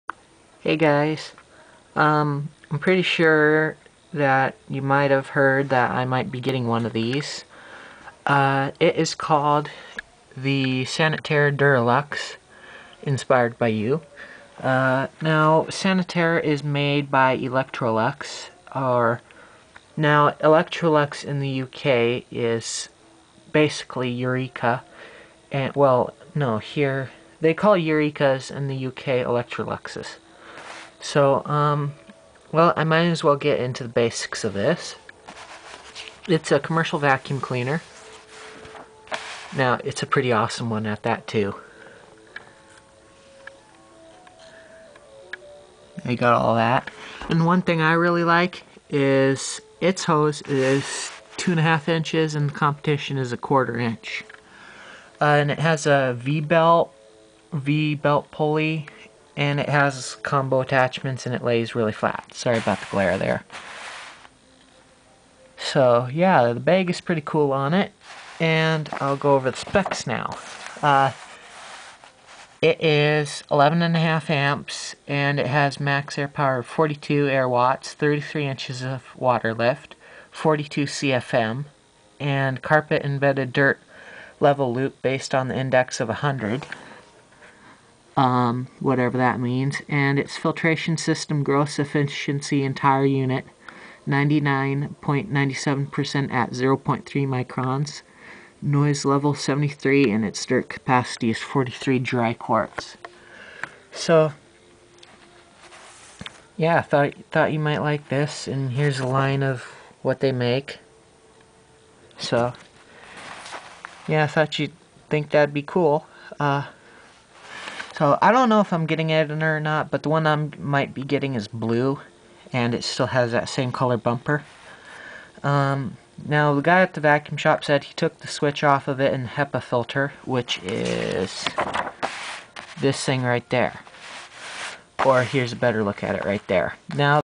a sneak peak of a new vacuum i might be getting in the future (read description)
i saw this in the vacuum shop window and i had to go check it out so i did and he had this little flyer on it so i took it home read it threw now i want to get it and then the other day went to go to that same vacuum shop to buy some cmf-1 filters and i asked him about it and he said he took the switch off it and the HEPA air filter so now i might get it for real cheep this sales for 500$ brand new by the way. so yeah i hope i can buy this vacuum and if you guys want me to take my camera to the vacuum shop when i go get it let me know in the comments ;)